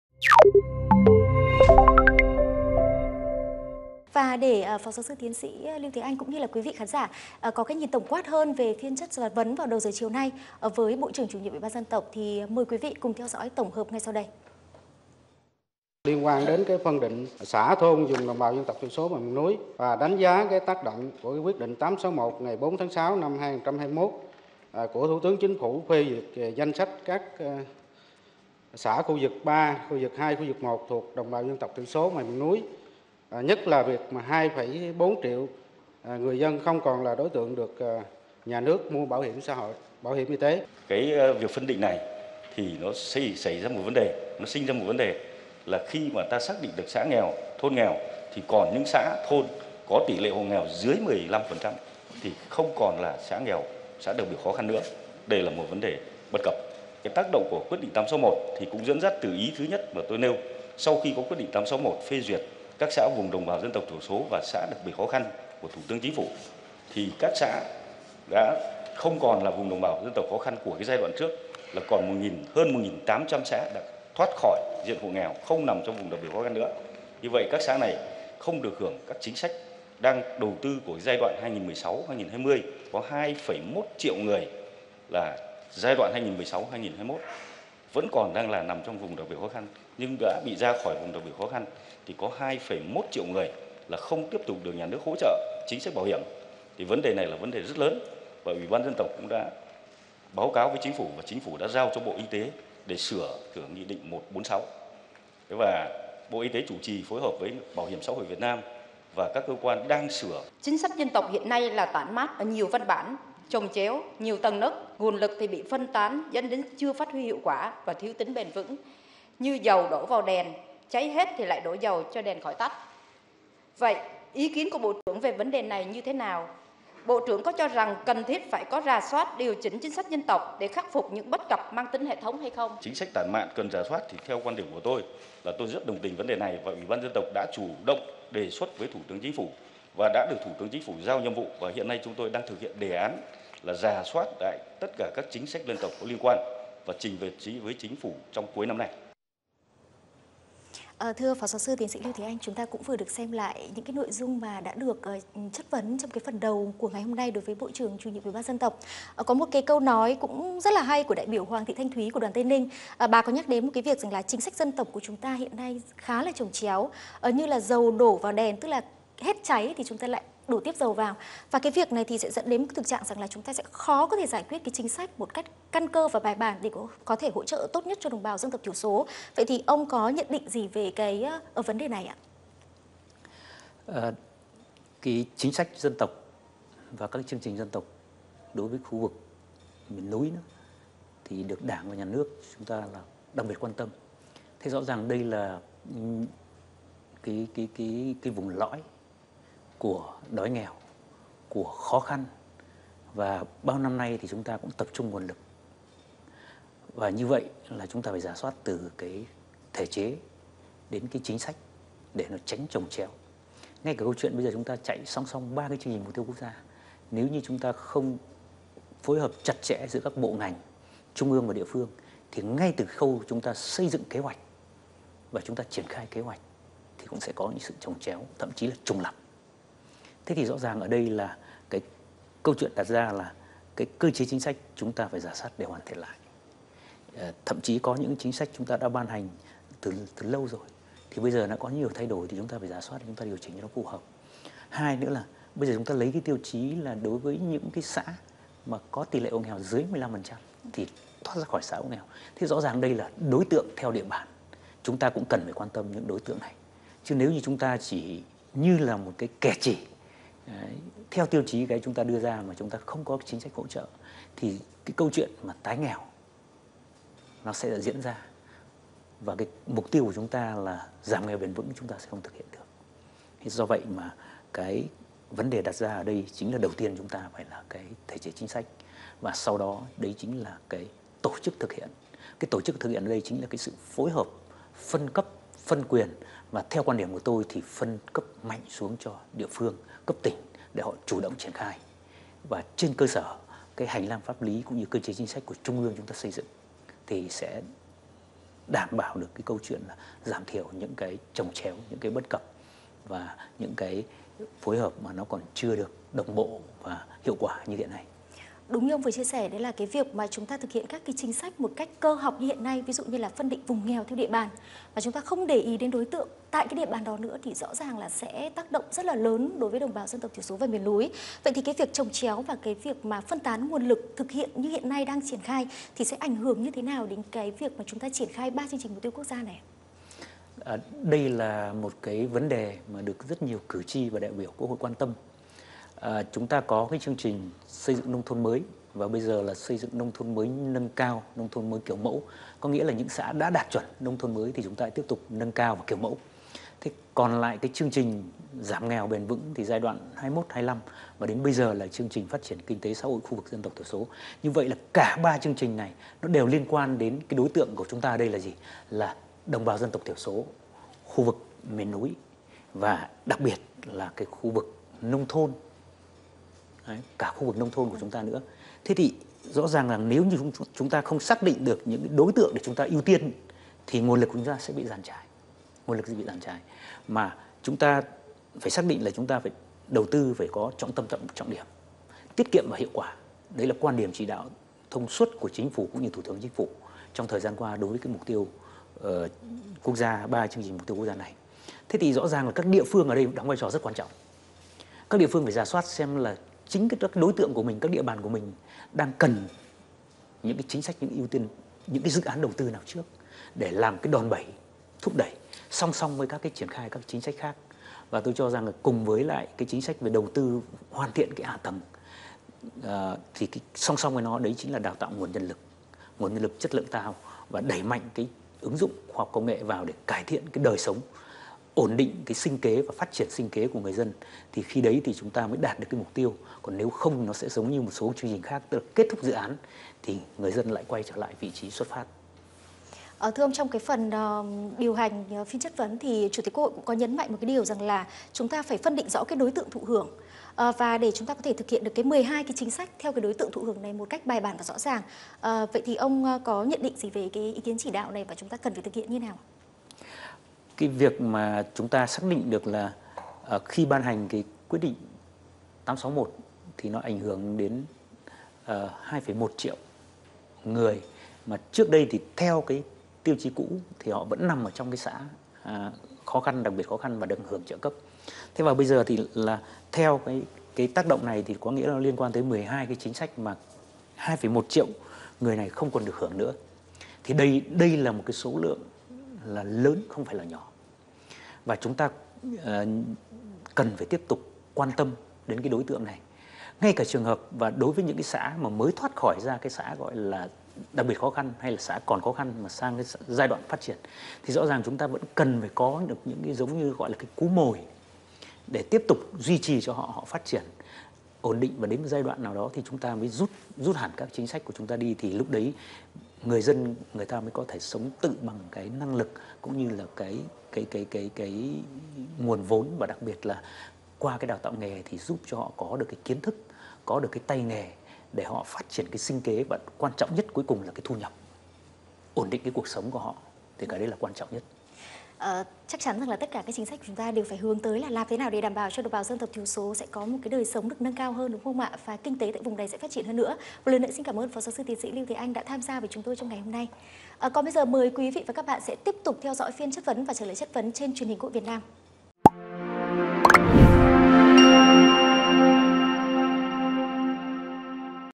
Hành lang Quốc hội: Chính sách dân tộc còn chồng chéo, khó đạt mục tiêu giảm nghèo bền vững
Truyền hình Quốc hội Việt Nam | Cùng bình luận về những vấn đề được các đại biểu Quốc hội chất vấn Bộ trưởng, Chủ nhiệm Uỷ ban Dân tộc Hầu A Lềnh về Chương trình mục tiêu quốc gia, THQHVN đã có cuộc trao đổi với ông Lưu Thế Anh, Viện trưởng Viện Tài nguyên và Môi trường.